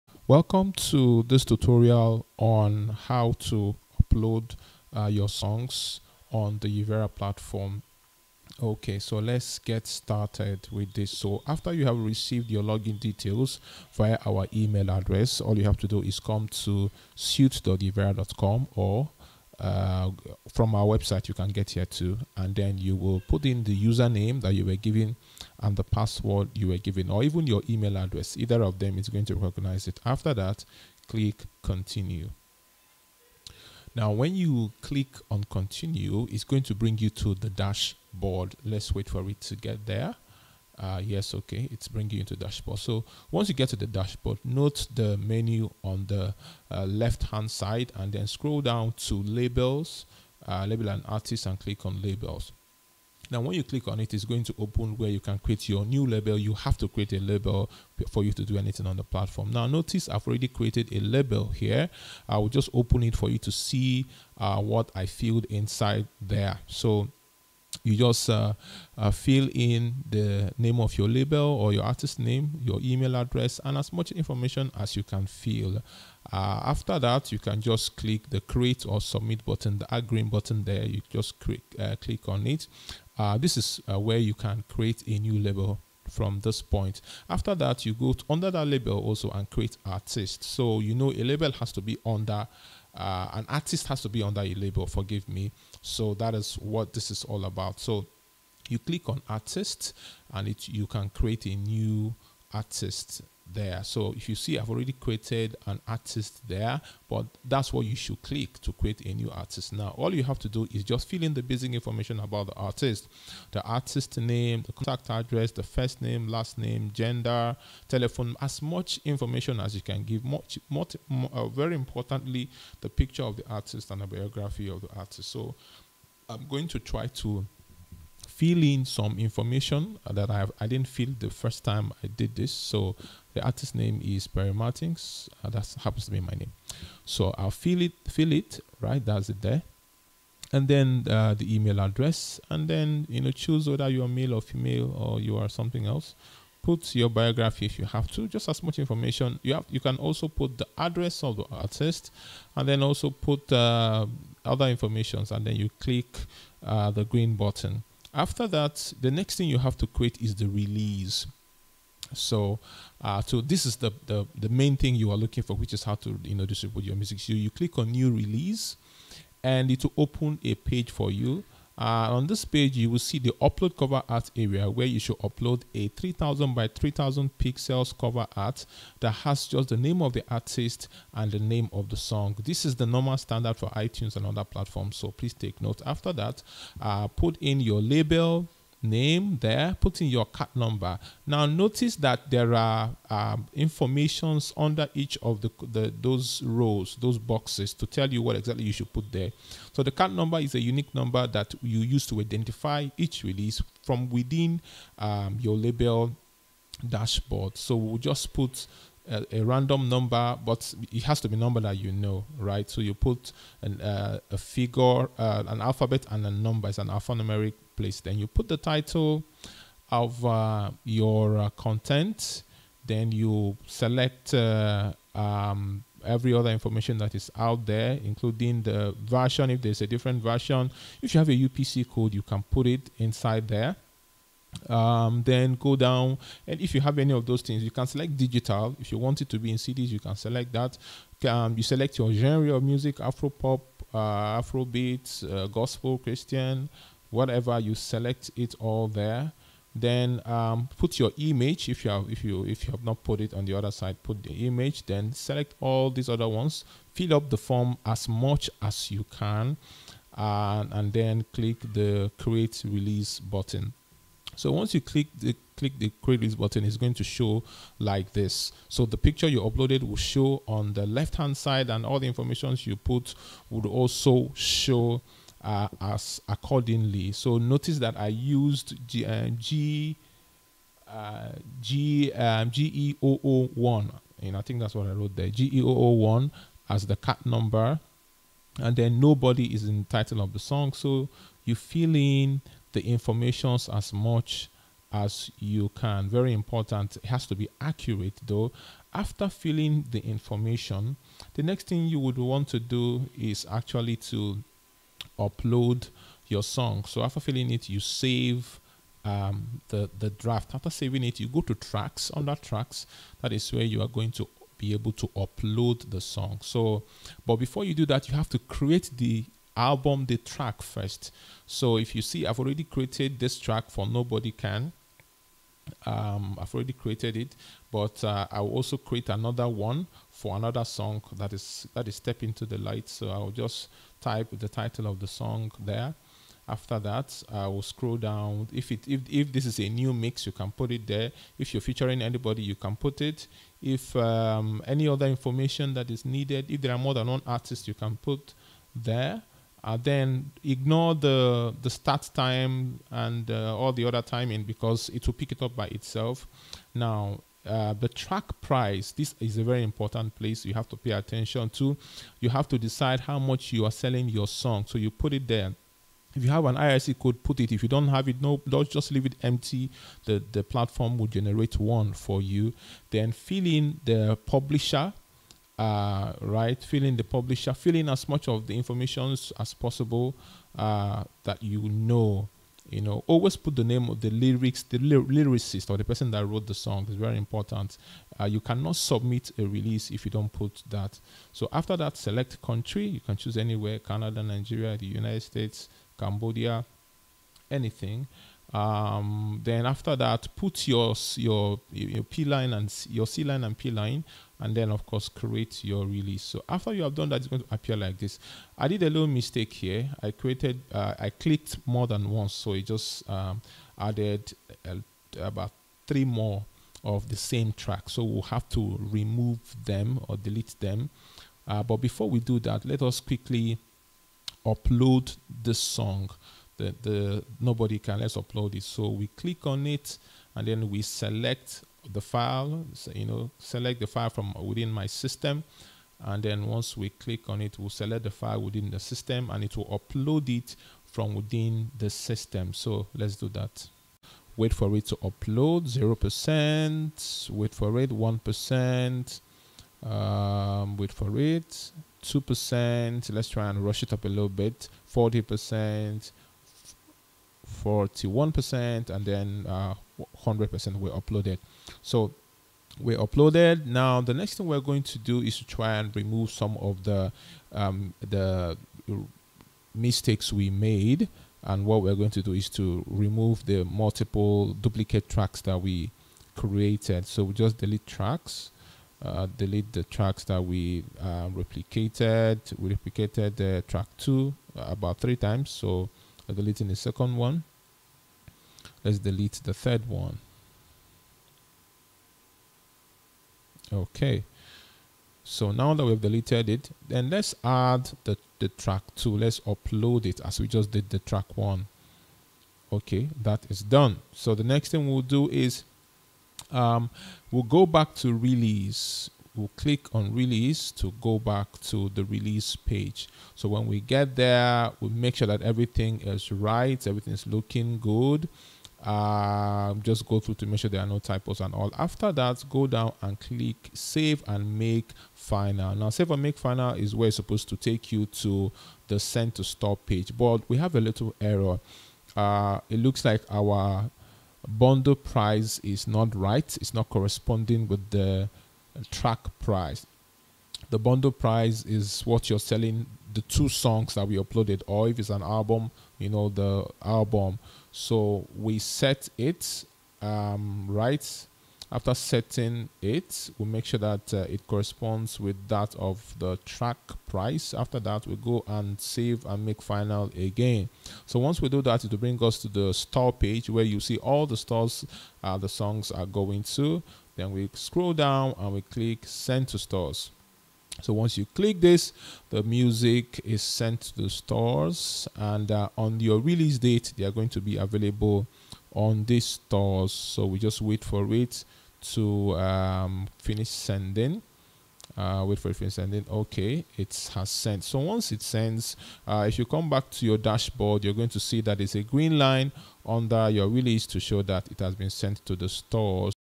Setting up your account and music release on Yivera tutorial
Setting up your account and releasing your music on Yivera tutorial.
If you have signed up on Yivera.com and have received your login details, then follow the steps in this video to set up your account and starting releasing your music to the stores.

Are you an African artist and you want to release your music to iTunes, Amazon, Spotify, Boomplay and over 150 other stores and platform?